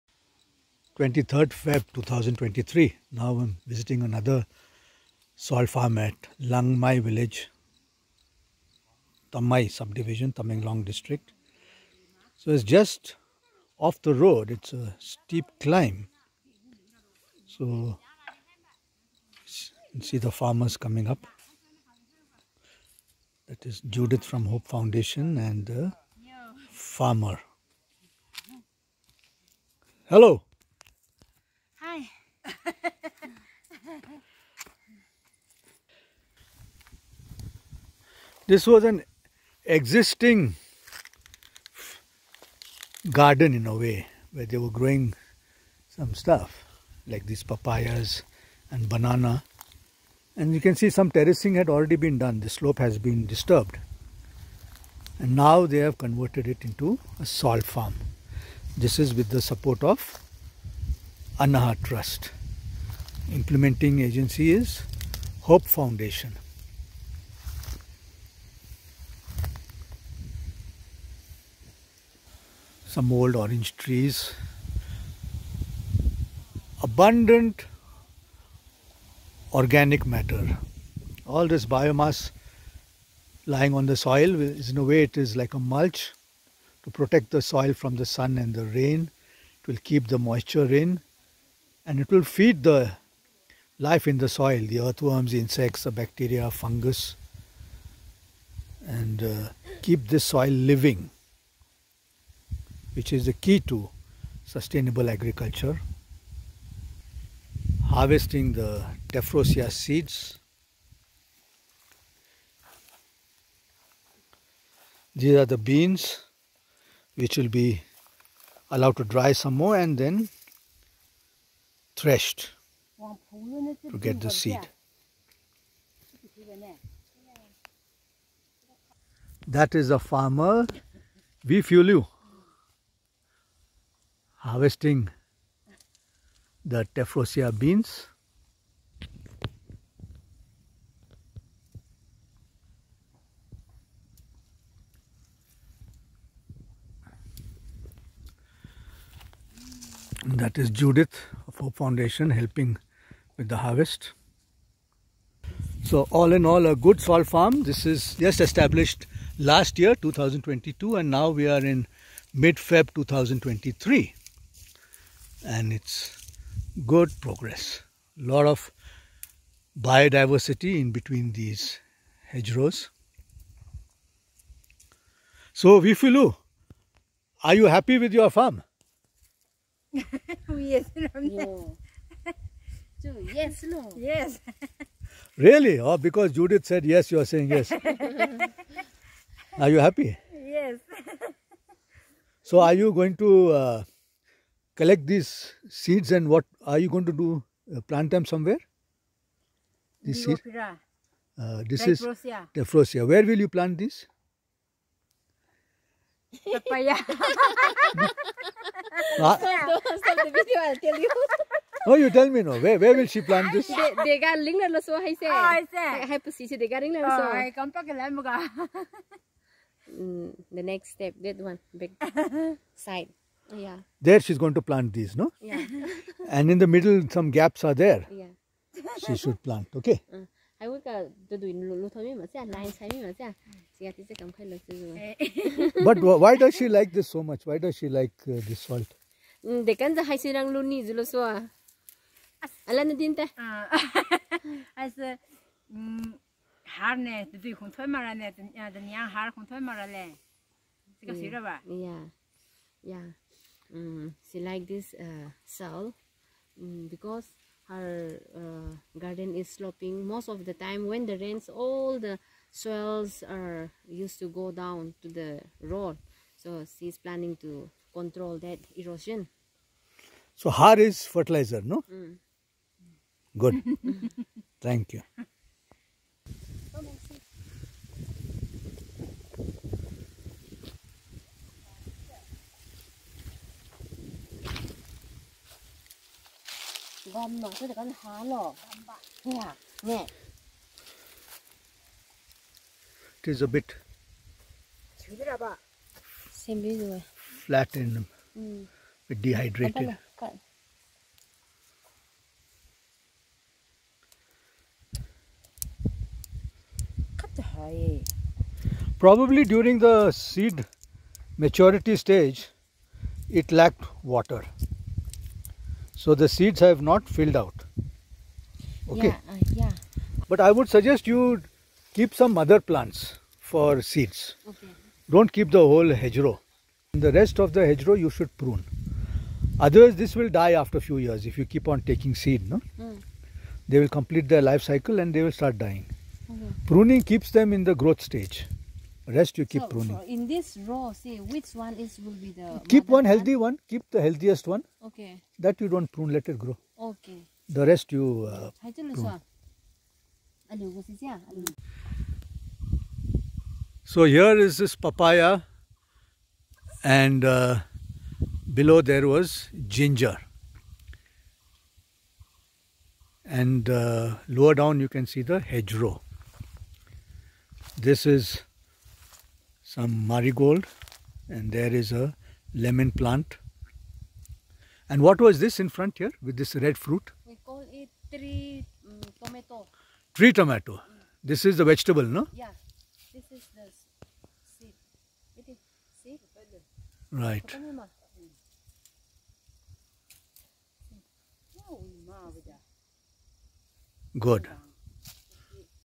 SALT Farm of Wiphiuliu Pamai, Tamei, Manipur - Feb'23
This video tour covers the hill garden of Wiphiuliu which she began converting into a SALT (Sloping Agriculture Land Technology) farm in 2022 by establishing few contour hedgerows of nitrogen-fixing species 'Tephrosia candida'. She is now harvesting the seeds of Tephrosia which she plans to use for gap-filling and establishing additional hedgerows on the farm for soil and water conservation and fertility management.

The SALT initiative at Tamei subdivision is being implemented on farmers' fields by Hope Foundation Society and funded by ANAHA Trust. The video commentary and recording is by David Gandhi on 23rd February 2023 at Langmei village.